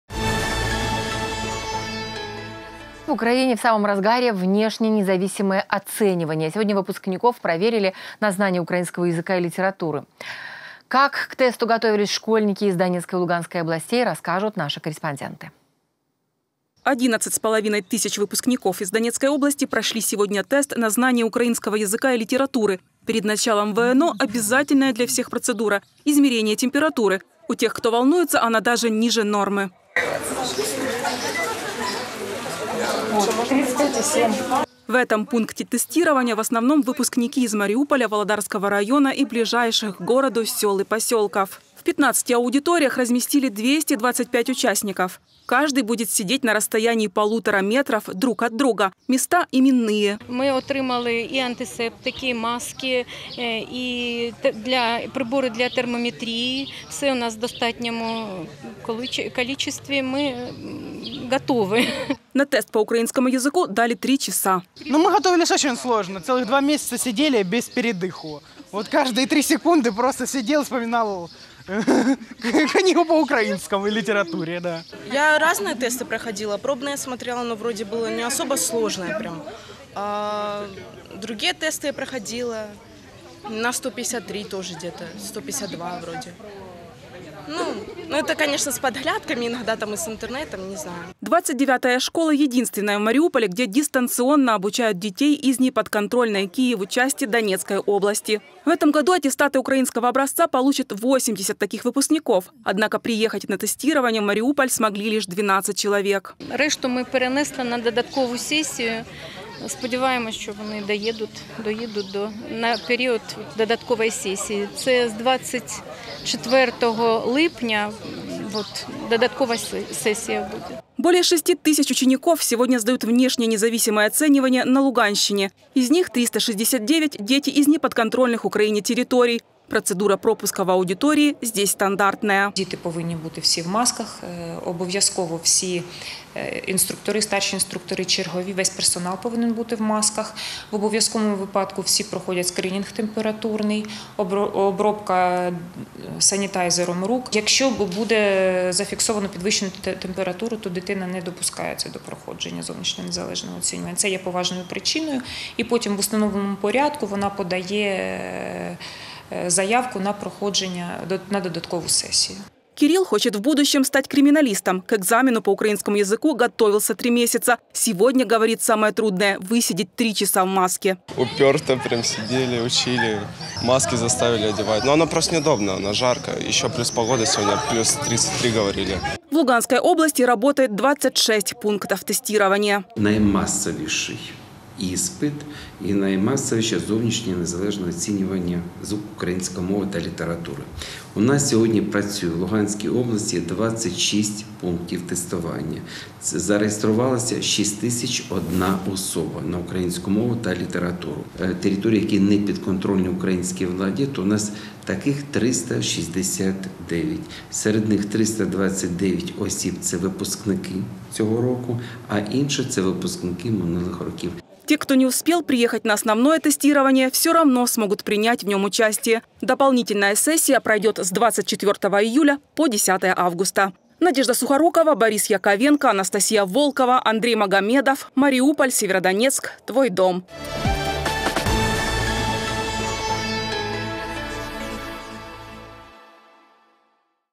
ВНО-2020: как проходил тест по украинскому языку и литературе на Донбассе
В Украине в самом разгаре Внешнее независимое оценивание. Сегодня выпускников проверили на знание украинского языка и литературы. Как к тесту готовились школьники из Донецкой и Луганской областей – расскажут наши корреспонденты.